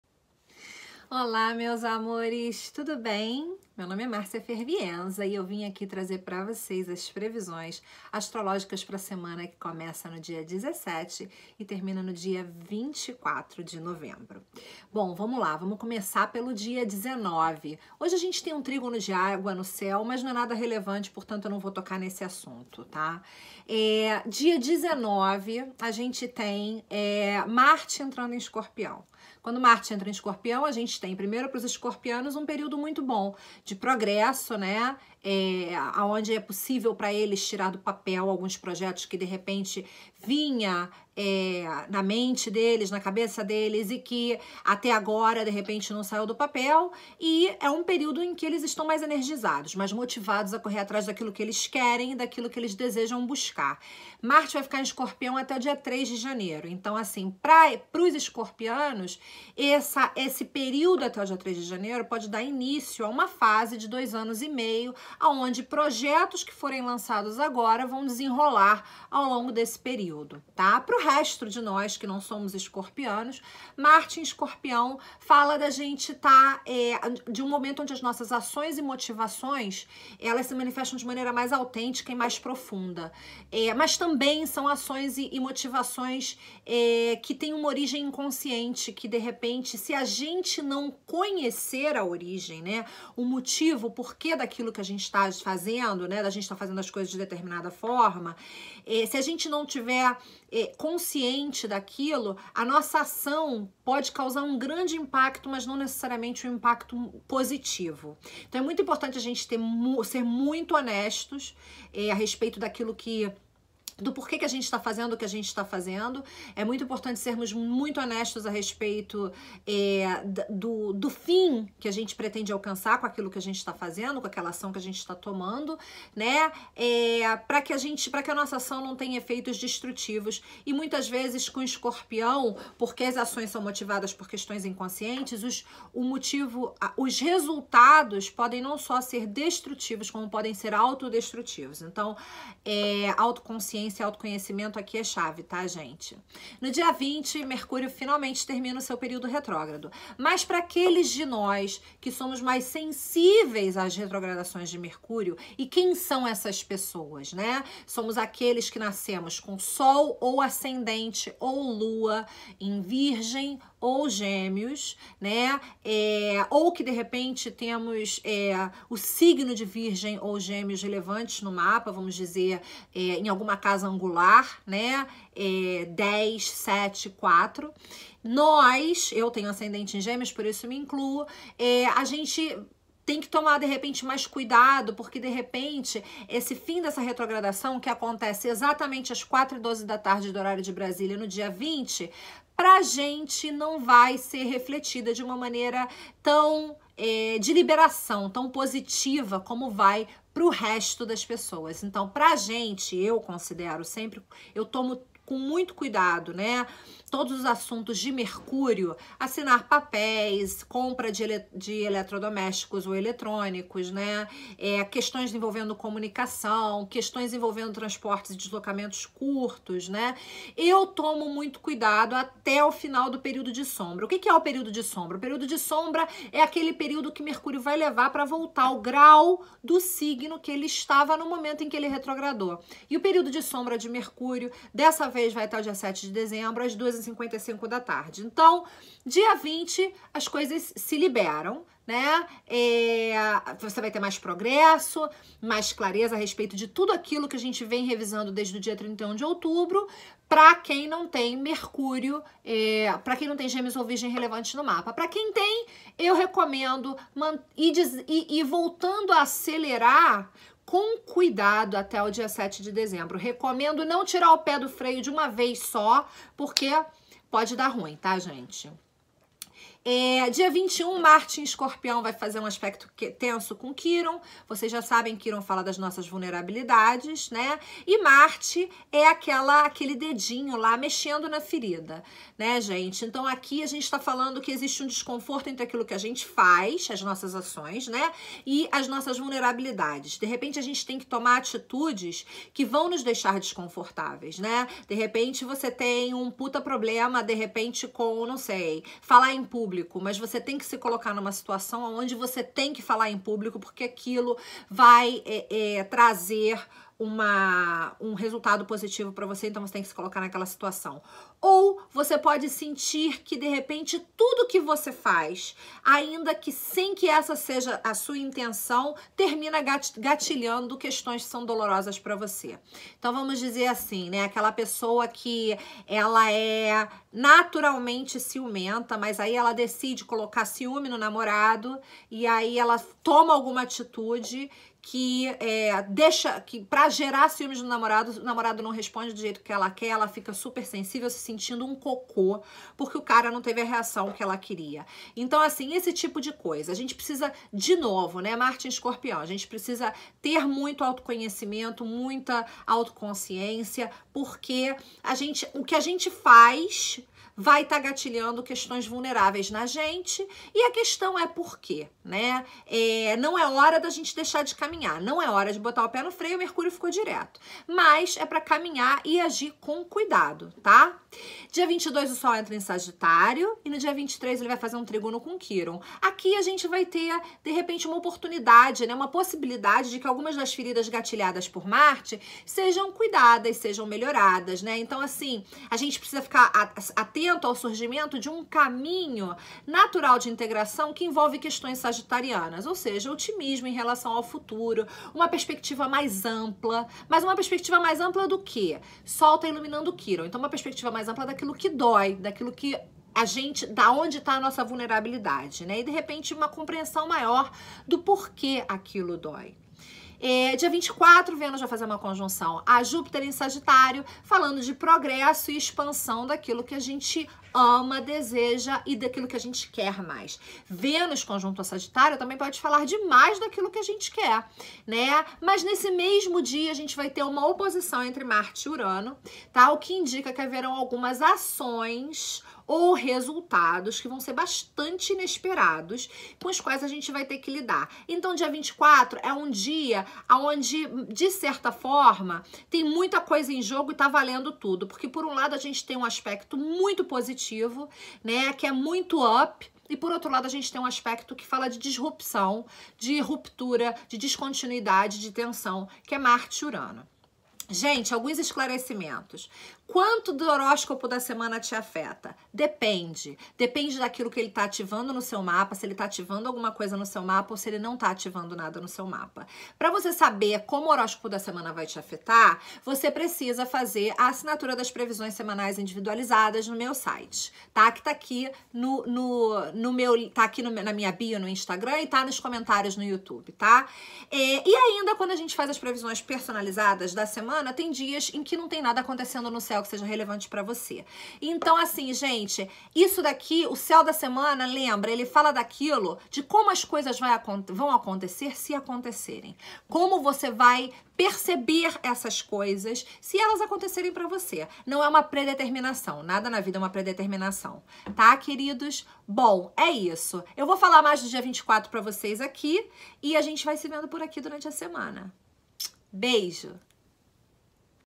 Previsões astrológicas pra semana de 17 a 24 de novembro
Mercúrio fica direto está semana! E temos o dia 24, que está quente.
Para receber estas previsões via e-mail, visite meu website e assine o newsletter